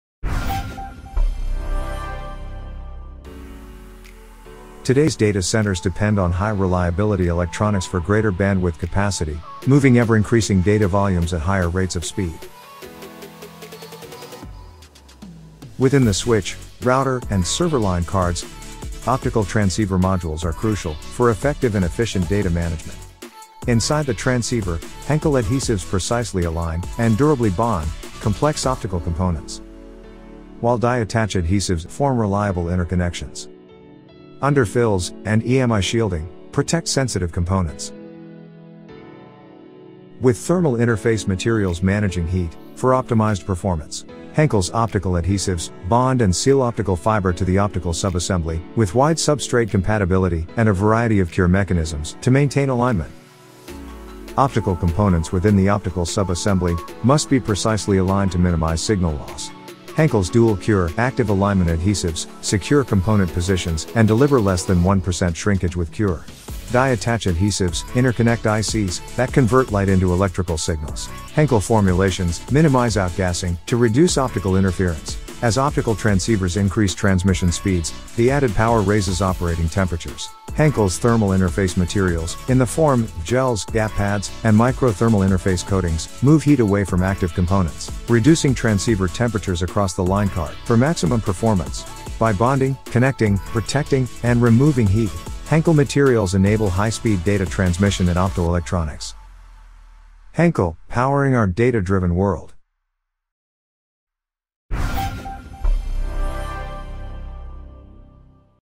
Henkel Adhesive Soland Thermal Management Solution for Optical Transceiver
Henkel material solutions for high performance optical trasnceiver applications. Discover how Henkel thermal management and adhesive solutions keep these devices reliably connected, protected and thermally controlled for optimized function.

More About Henkel Data & Telecom Infrastructure Solutions

Connect with Henkel Adhesive